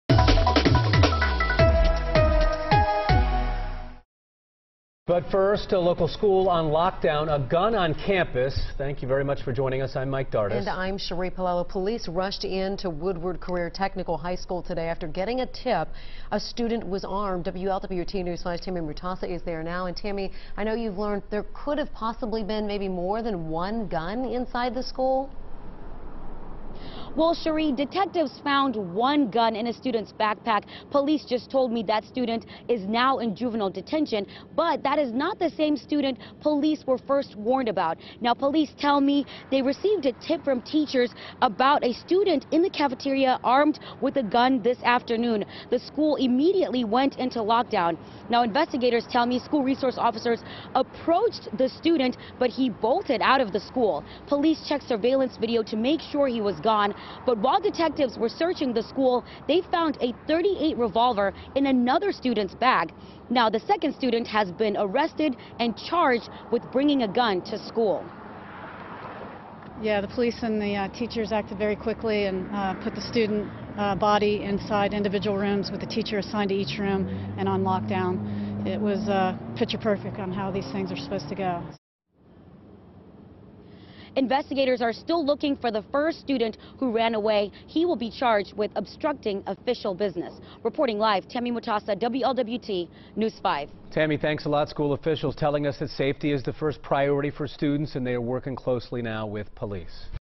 Student sought after gun found at Woodward H.S.
Cincinnati police are looking for a student after a weapon was found at Woodward Career Technical High School on Monday. Police responded to the school shortly after 1 p.m. after officials got a tip from the school that a student was going to have a Subscribe to WLWT on YouTube now for more